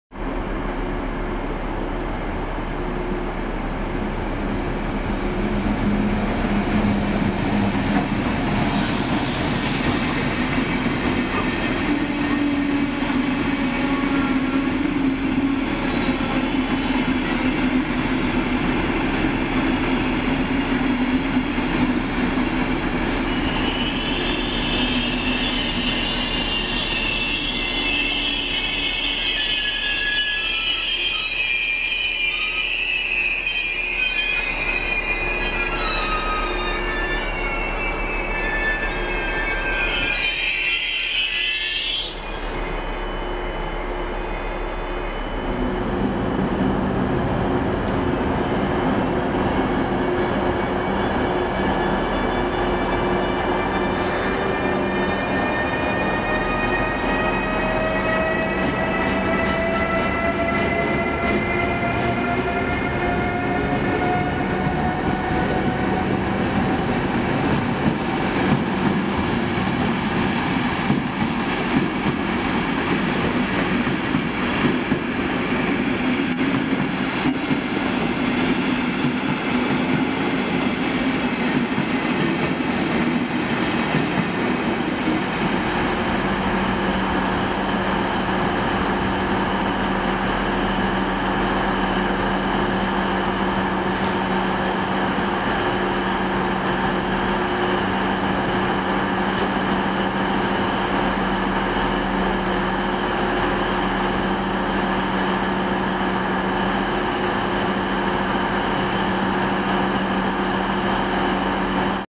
Der "neue" ALEX Nord in Regensburg
Mit dem 10.12.17 trat auch ein neuer Übergangsvertrag der BEG auf dem ALEX Nord in Kraft. Dieser sieht u.a. alle 2h eine Verbindung von/nach Prag vor. Die Hofer und Prager Zugteile  werden dabei in Schwandorf geteilt/vereinigt. Außerdem kommen neu modernisierte UIC-Z1-Wagen vom Netinera-Mutterkonzern Trenitalia zum Einsatz. In dieser Konstellation kam der ALX 84108/358 am 15.12.17 in Regensburg an und setzte nach dem Lokwechsel auf die MGW-183 500 seine Fahrt nach München fort - leider mit ordentlich Verspätung.

The ALEX-service Hof/Prague - Munich at Regensburg Central on 15th december 2017.

© ICE 91 Prinz Eugen 2017